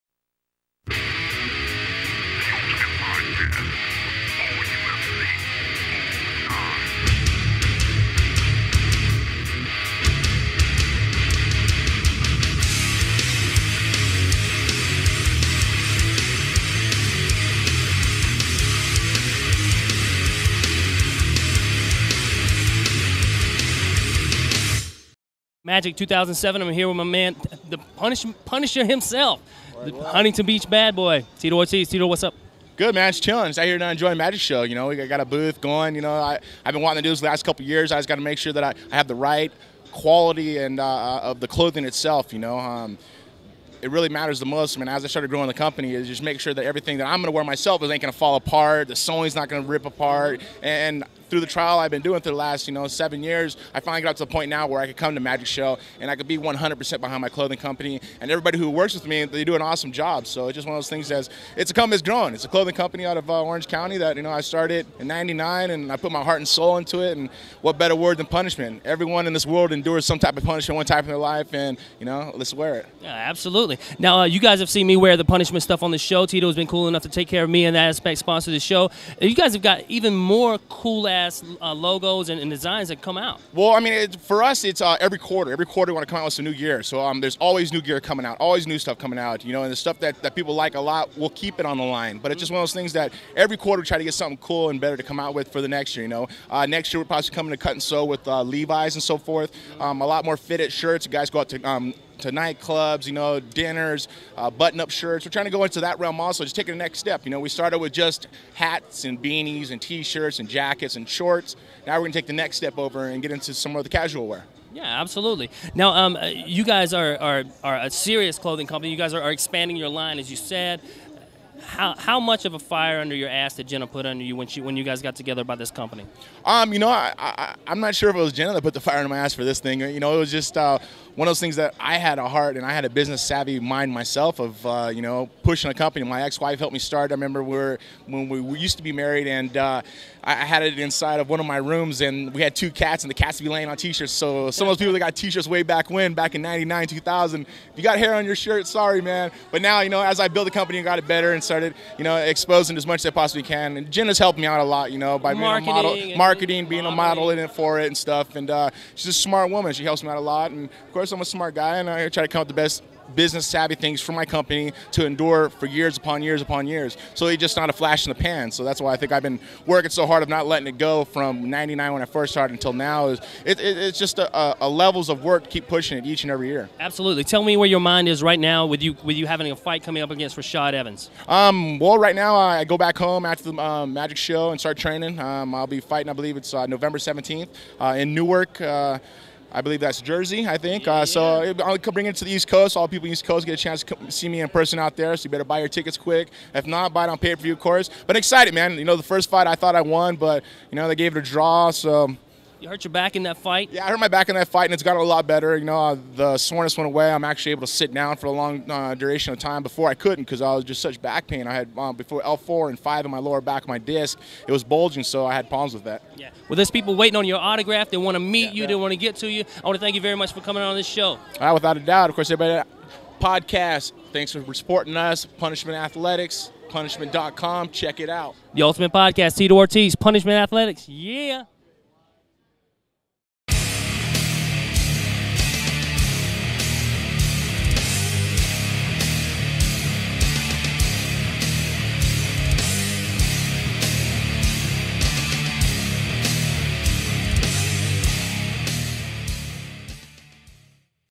the Ultimate Podcast Gary Alexander and Tito Ortiz
Tito Ortiz joins Gary Alexander here and talks UFC, MMA, Punishment, Jenna and Rashad Evans!